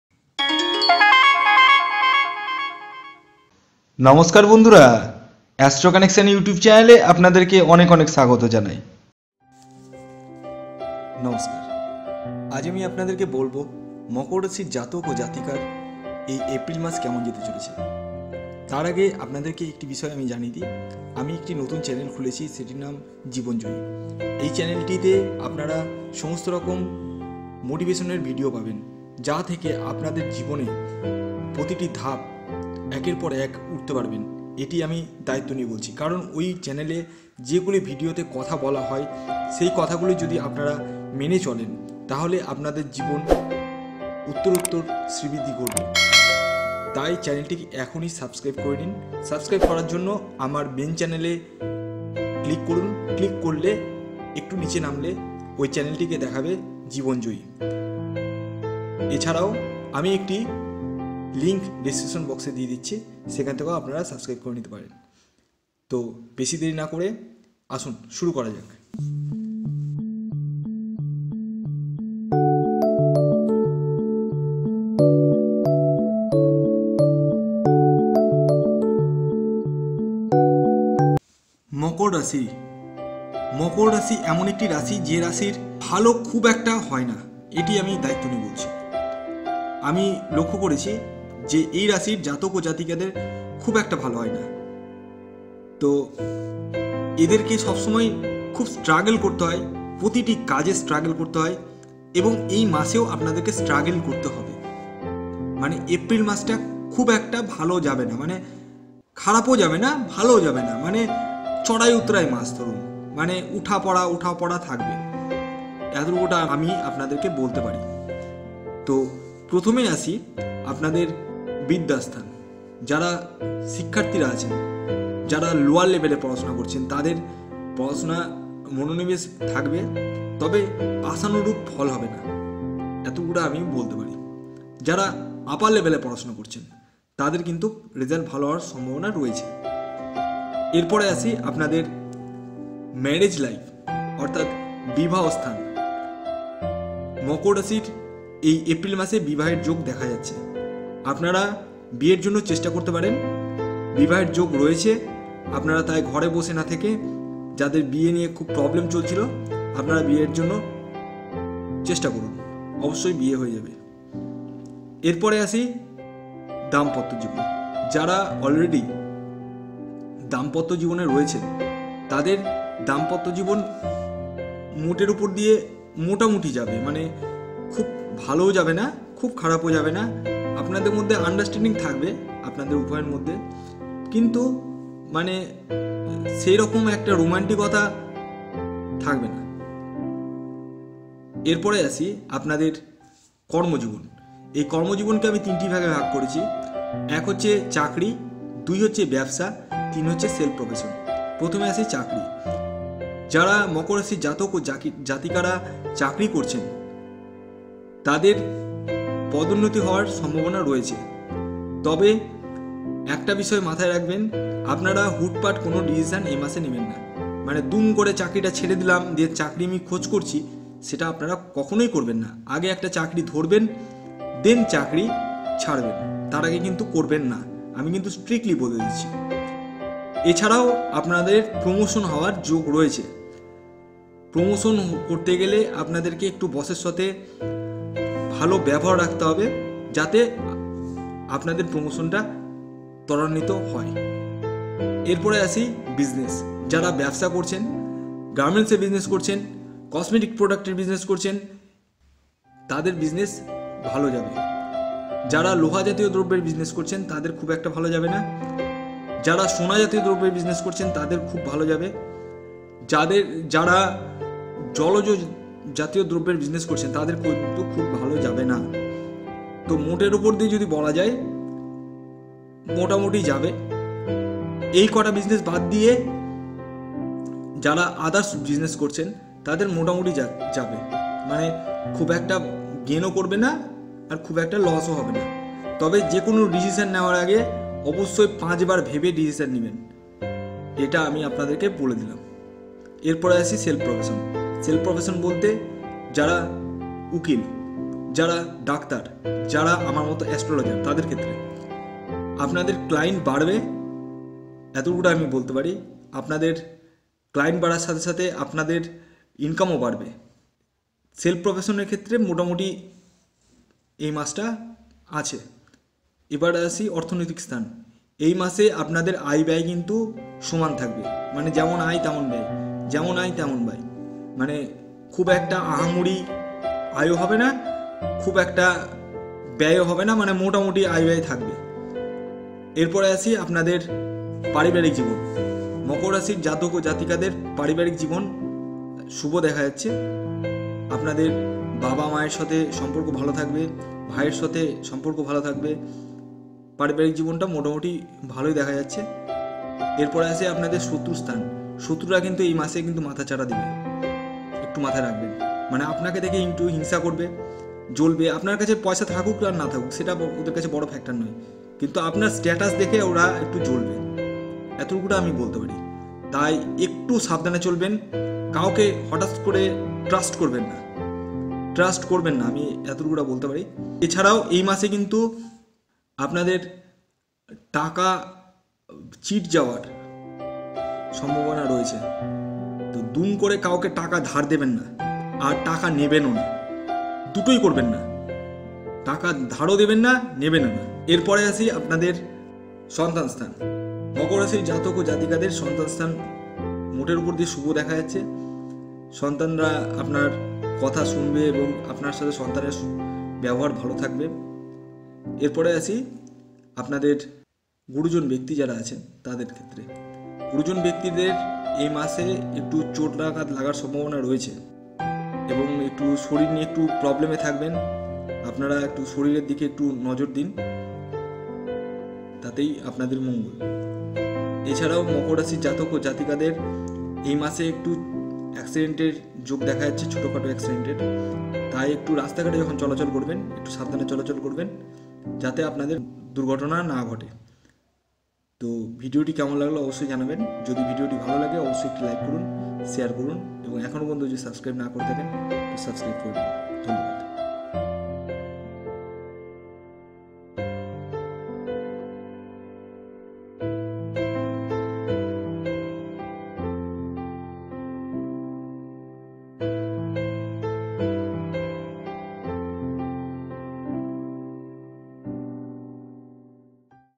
মকর রাশির এপ্রিল মাস চড়াই-উতরাইয়ের মাস।
বন্ধুরা এই ভিডিওতে আমি বলেছি মকর রাশির জাতক-জাতিকাদের এপ্রিল মাস কেমন যেতে চলেছে। এই মস চড়াই-উতরাইয়ের মাস।